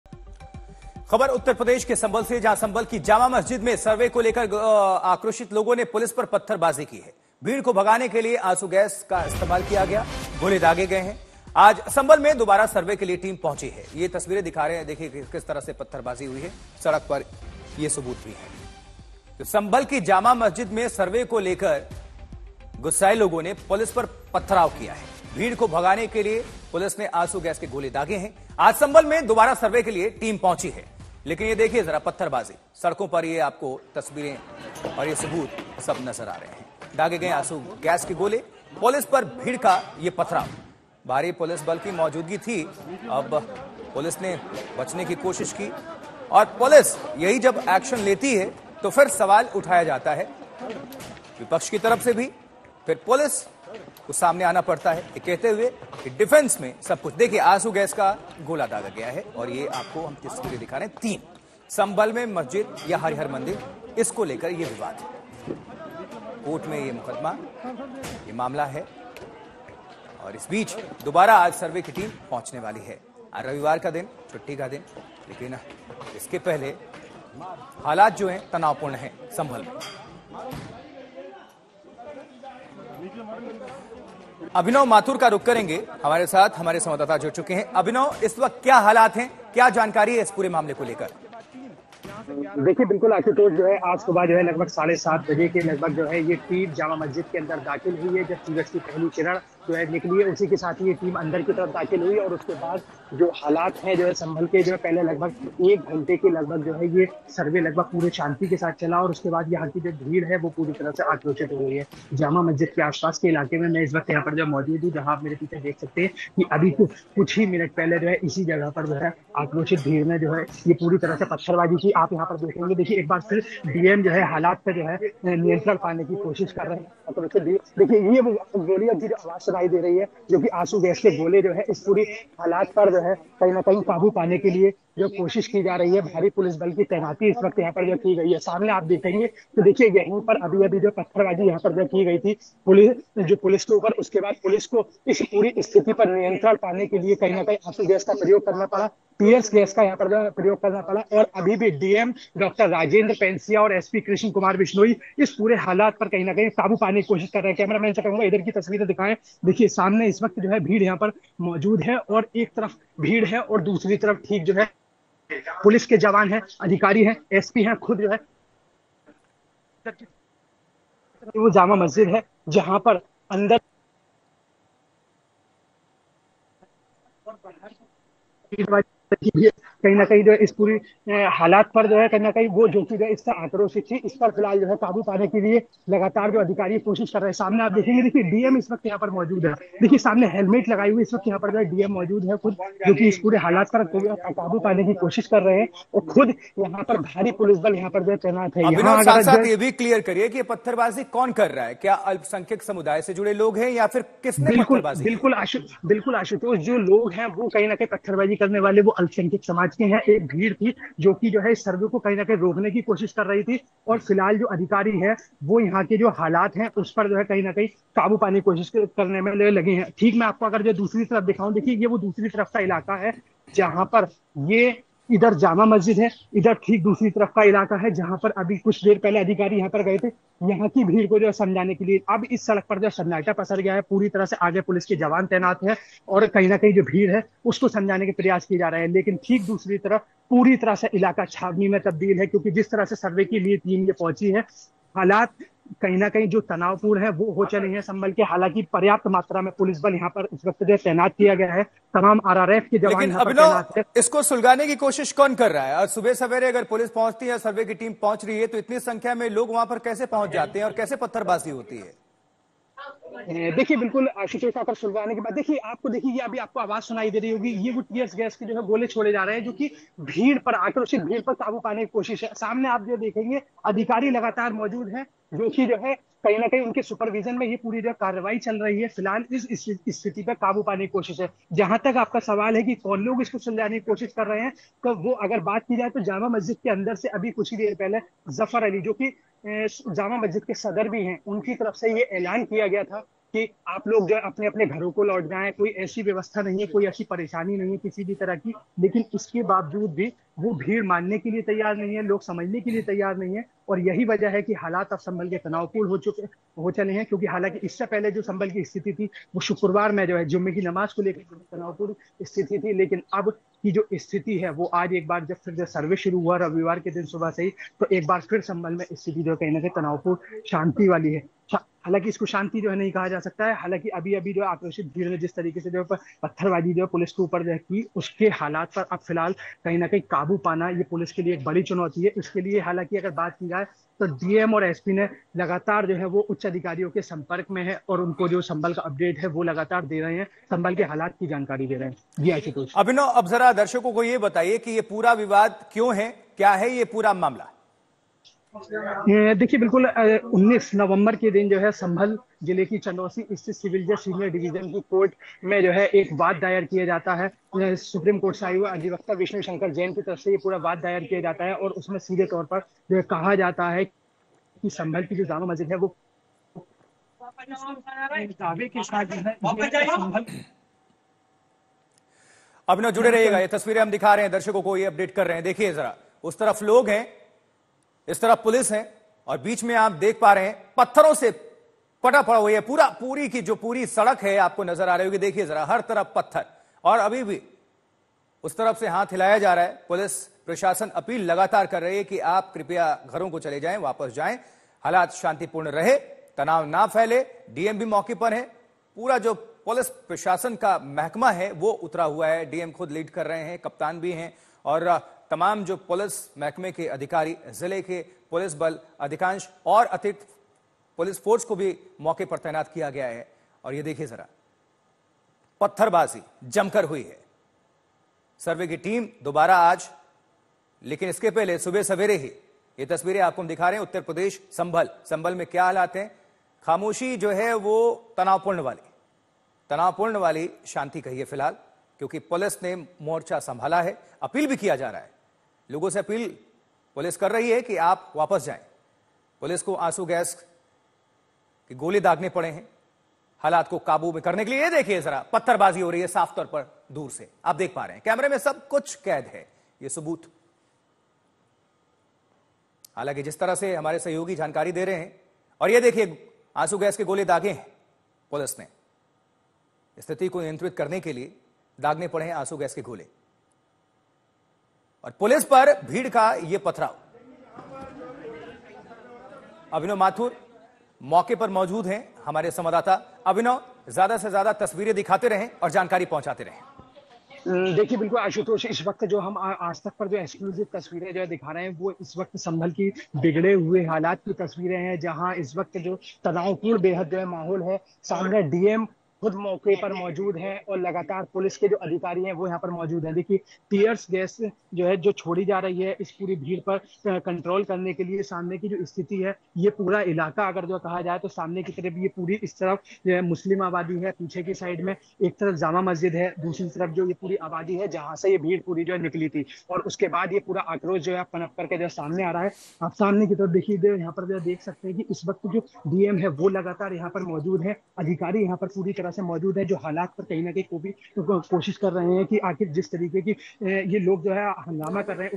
Breaking News: Sambhal की Jama Masjid में सर्वे के दौरान पथराव और फायरिंग, उग्र भीड़ ने फूंकी कार
संभल की जामा मस्जिद में सर्वे को लेकर गुस्साए लोगों ने पुलिस पर पथराव किया है. भीड़ को भगाने के लिए पुलिस की तरफ से आंसू गैस के गोले दागे गए हैं. कोर्ट के आदेश के बाद जब आज दूसरी बार सर्वे करने के लिए टीम फिर से पहुंची तो भीड़ भड़क गई और भीड़ ने पुलिस पर हमला कर दिया.